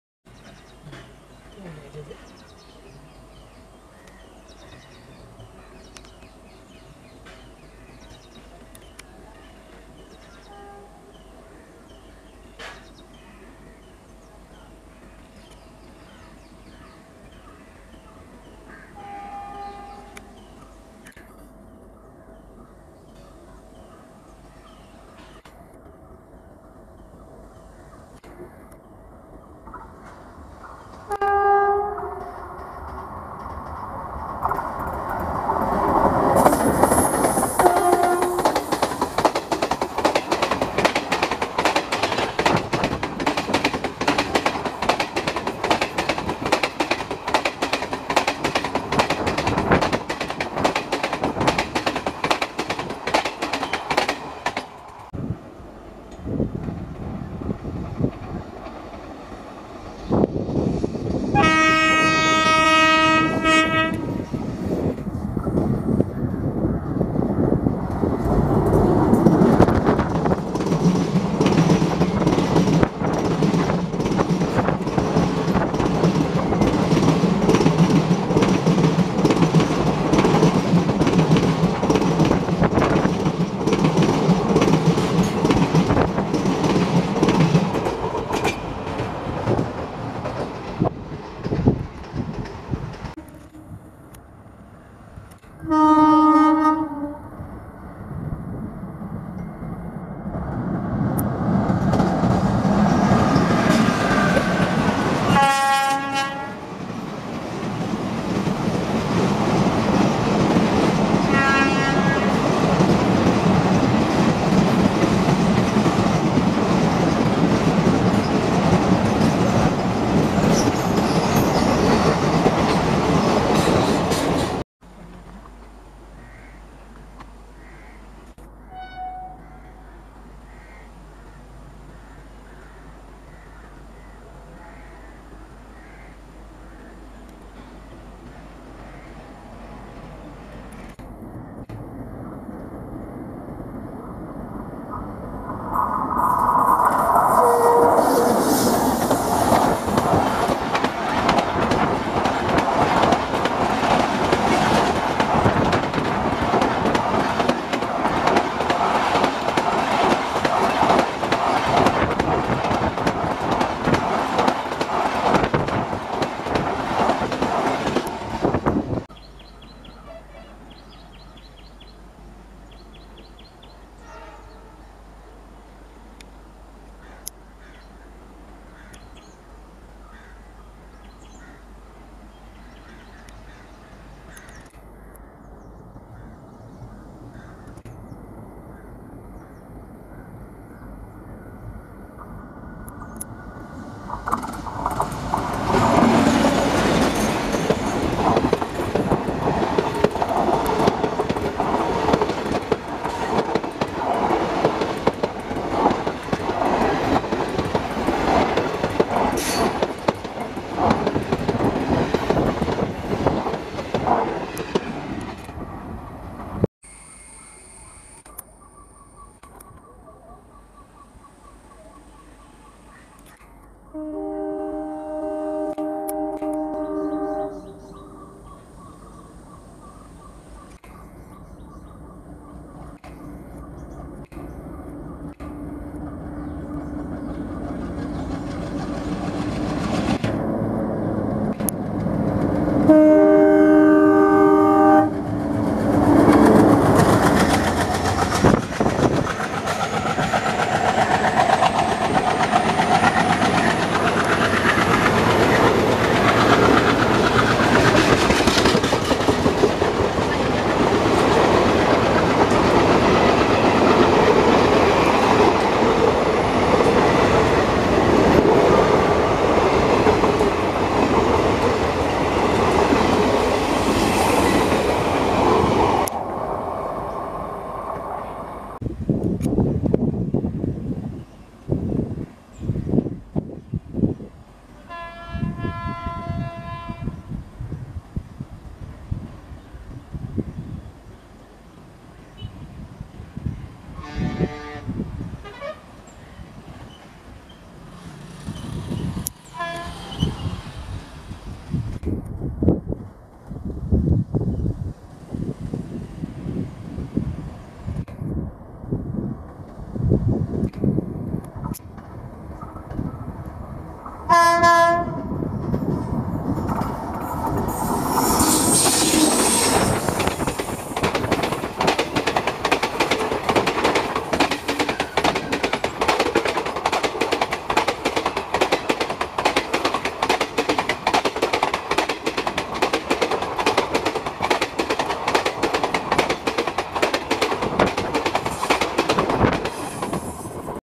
7 in 1 | Route diverted and regular train compilation | congestion | Southern Railways
INDIAN RAILWAYS

12076/Thiruvananthapuram Central - Kozhikode Jan Shatabdi Express
തിരുവനന്തപുരം സെൻട്രൽ - കോഴിക്കോട് ജന ശതാബ്ദി എക്സ്പ്രസ്     तिरुवनंतपुरम सेन्ट्रल - कोषिक्कोड जन शताब्दी एक्सप्रेस

16326⇒22646/Ahilya Nagari SF Express (PT)
اھیلیانگری سوپرفاسٹ ایکسپریس     अहिल्या नगरी सुपरफास्ट एक्सप्रेस

22640/Alappuzha - MGR Chennai Central (Alleppey) SF Express (PT)
ആലപ്പുഴ - ചെന്നൈ സെൻട്രൽ അലപി സൂപ്പർഫാസ്റ്റ് എക്സ്പ്രസ്സ്‌     आलप्पुष़ा - एम० जी० आर० चेन्नै सेन्ट्रल अल्लेप्पी सुपरफास्ट एक्सप्रेस

12626/Kerala Express (PT)
ਕੇਰਲਾ ਐਕਸਪ੍ਰੈਸ / کیرلا ایکسپریس     केरला एक्सप्रेस

06015/Ernakulam Jn - Velankanni (Via Kollam Jn) Summer Special (PT)
എറണാകുളം ജംഗ്ഷൻ-വേളാങ്കണ്ണി സമ്മർ സ്‌പെഷ്യൽ (കൊല്ലം വഴി)     एरणाकुलम जंक्शन - वेलांकन्नि ग्रीष्मकालीन विशेष ( कोल्लम होकर )

19262/Porbandar - Kochuveli Weekly Express (PT)
પોરબંદર --કોચુંવેલી સાપ્તાહિક ઍક્સ્પ્રેસ     पोरबंदर - कोच्चुवेली साप्ताहिक एक्सप्रेस

17229/Sabari Express (PT)
ശബരി എക്സ്പ്രസ്സ്‌ / శబరి ఎక్స్‌ప్రెస్     शबरी एक्सप्रेस